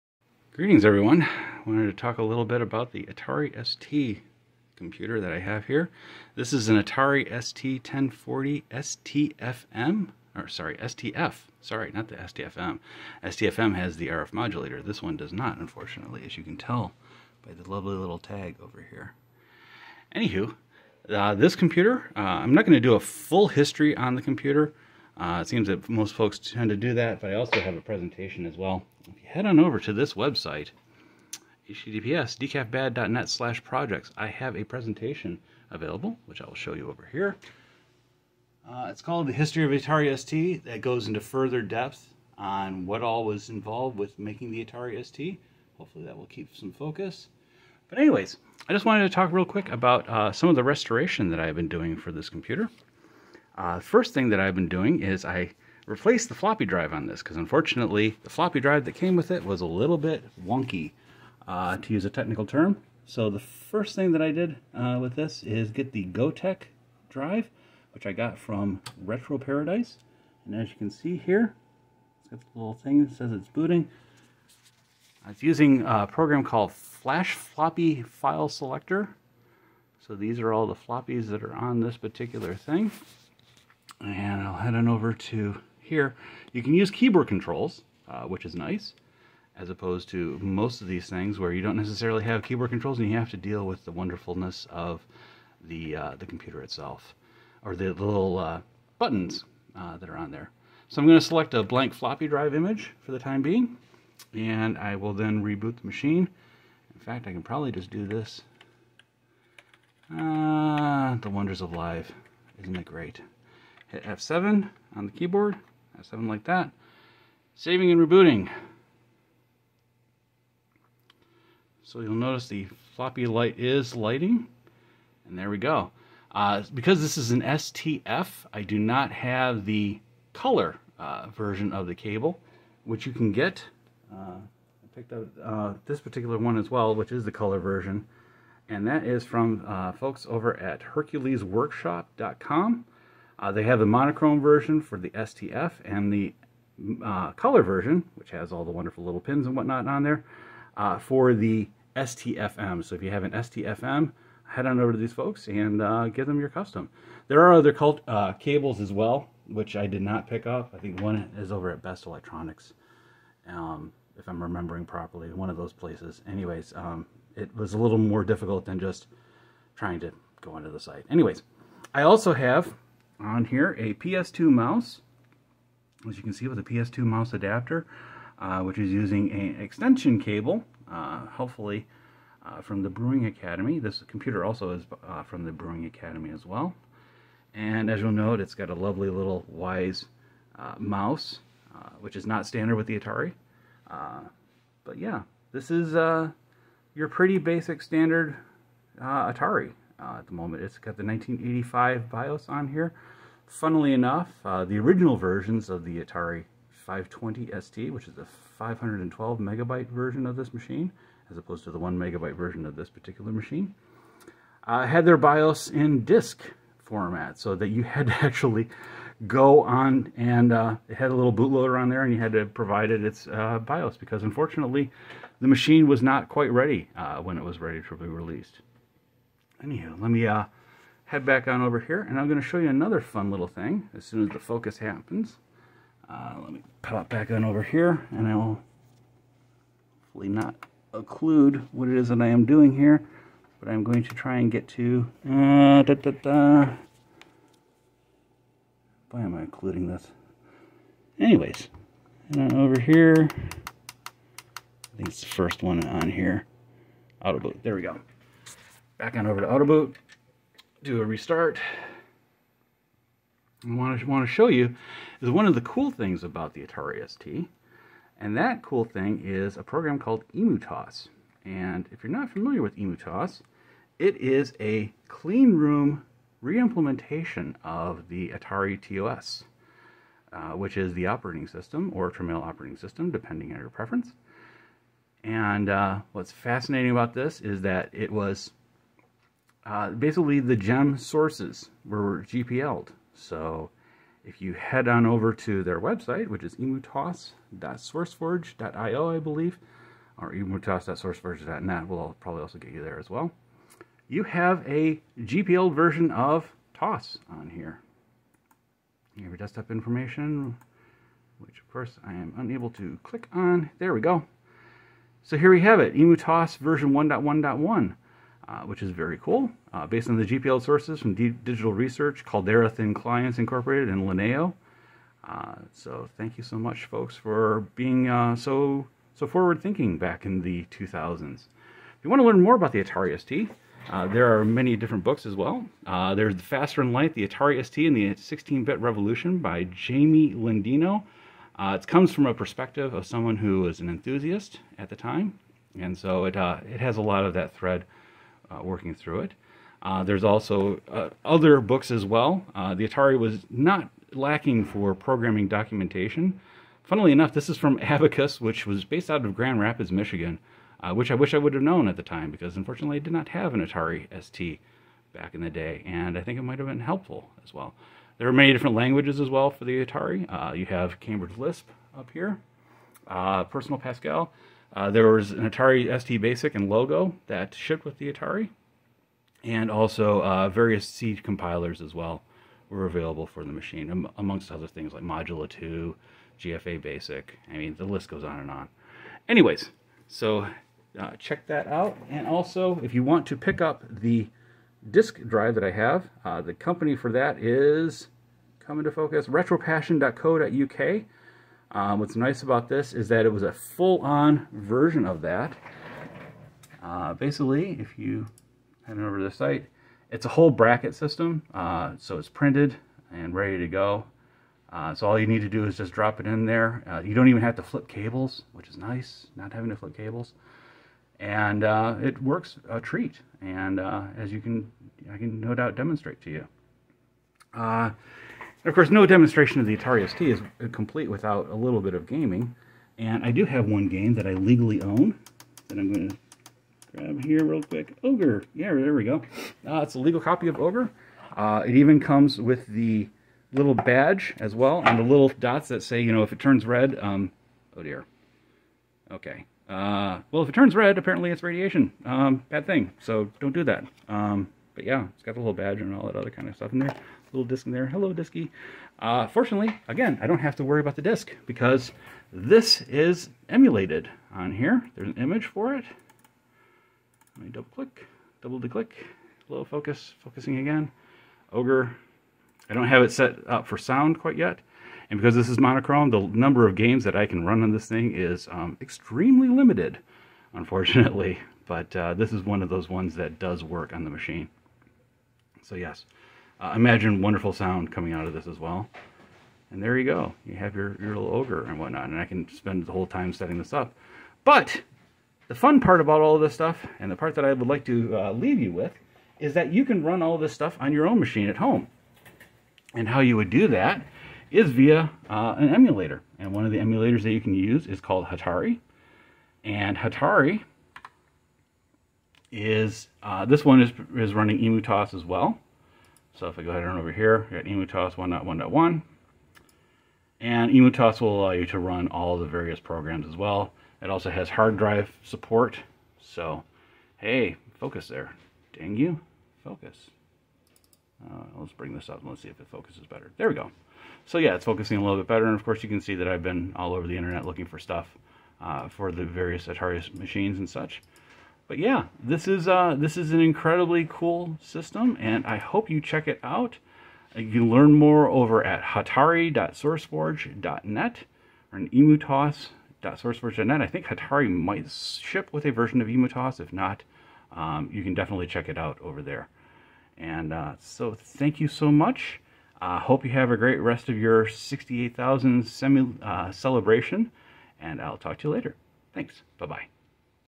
Quick demonstration of the Atari 1040STF
This is a quick demonstration of the Atari 1040STF with a Gotek drive as well as the extension cables and PS/2 adapter.